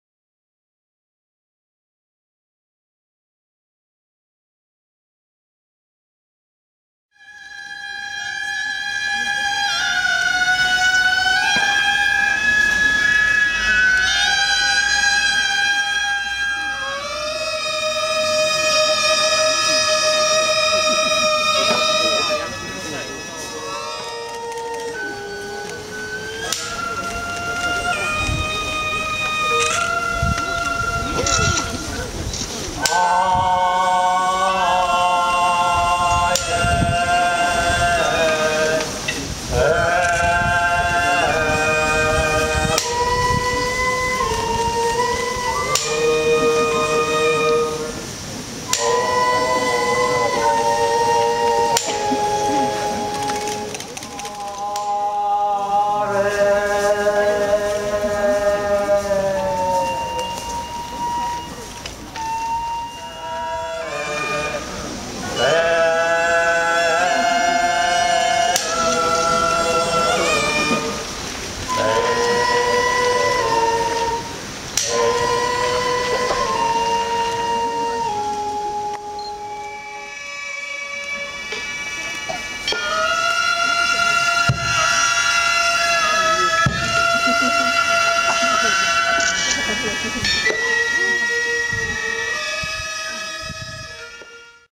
下鴨神社 御蔭祭 切芝神事 東遊
下鴨神社の御蔭祭で行われた切芝神事での東遊の様子です。枕草子でも「いとおかし」と評された優雅な舞です。